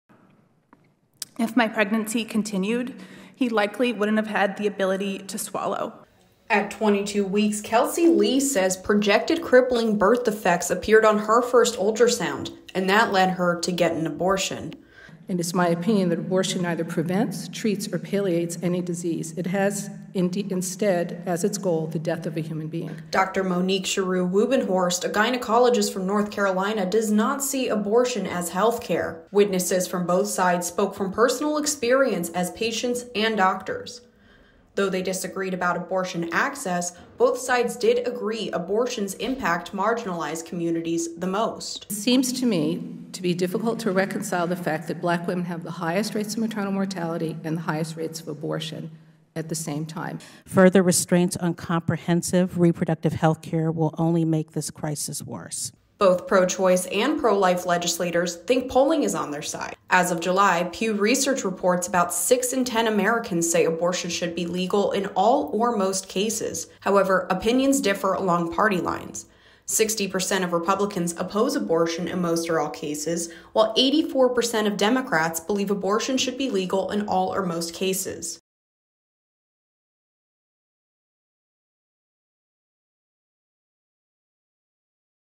Democrats examine the harm of Republican-backed abortion bans
WASHINGTON - Since Roe v. Wade was overturned in June, 15 states have banned abortion entirely. New York Rep. Carolyn Maloney is holding a hearing today about the impact of abortion bans on women nationwide, with a focus on whether or not to define the procedure as healthcare.